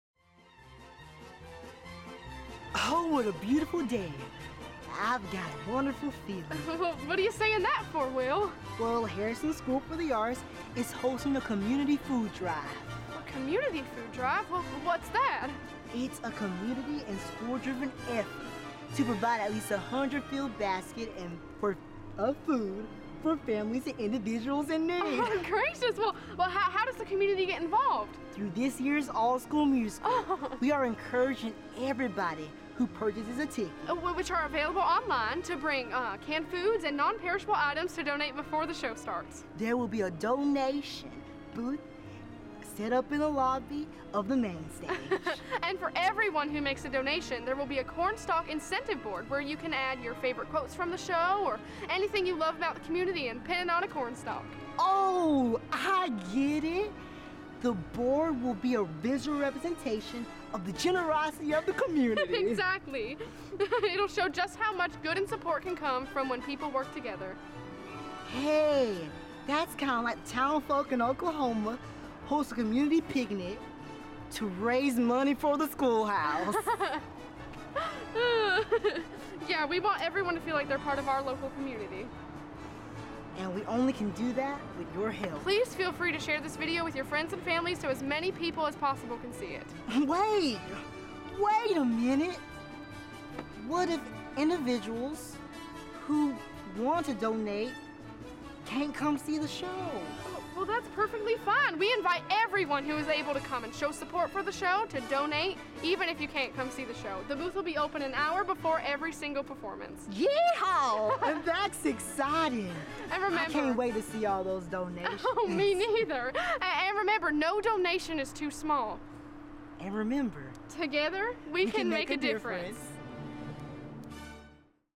HSA Food Drive
Harrison School for the Arts Community Food Drive October 31-Nov. 3.